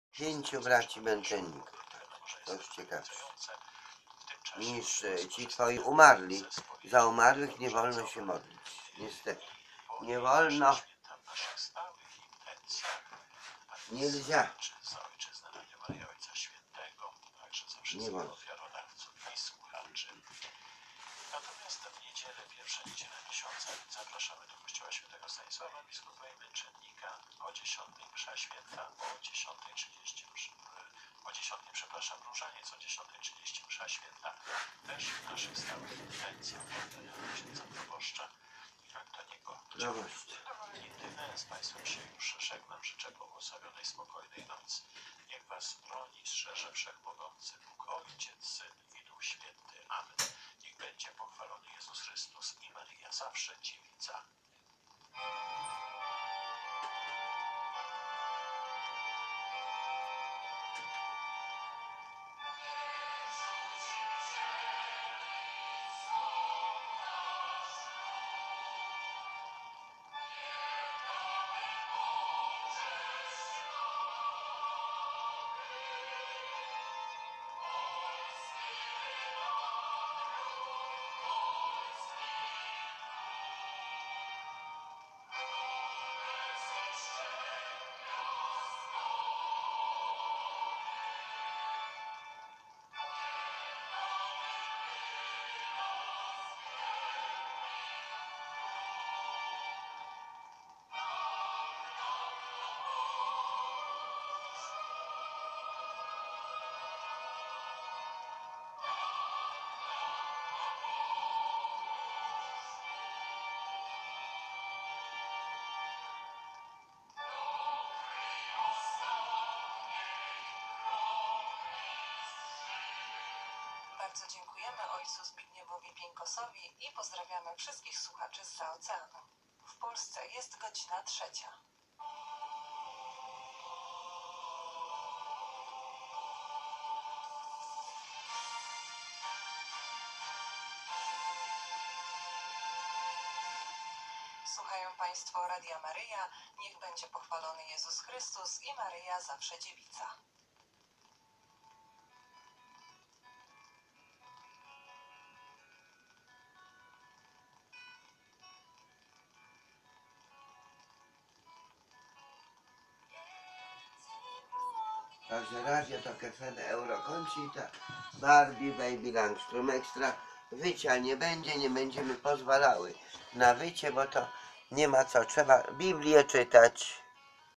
W WA-WIE/PONOĆ JAK NA Z. MA BYĆ/ALE OPORY SĄ OGROMNE POŚRÓD DEWOCTWA STARYCH KRYŚ ZELEWSKIEGO!!
AHA//ŻE//TAK!!JASNE!! NO,ALE TY SIĘ KRYCHA ZASTANÓW DLACZEGO JAŚKO WYSYŁA TĘ DEWOTKĘ MICHAŁA DO WIĘZIENIA,SKORO TAM BYŁ I ŻE PO CO MIAŁBY TAM ZNÓW WRACAĆ!?TACY LUDZIE PRZYCHODZĄ NA WERNISAŻE! ALE PO CO KOMU TAKA MUZYKA W STYLU IWONY JĘDRUCH,SKORO JAŚKO Z SASKIEJ POWIEDZIAŁ,ŻE PRYMITYWNA MUZYKA I PRYMITYWNE ZAJĘCIE,ŻE BABA ŻERUJE NA GŁUPOCIE LUDZI,BO ONI JEJ PŁACĄ EWENTUALNIE,A TU TAKIE WALENIE W GONGI!?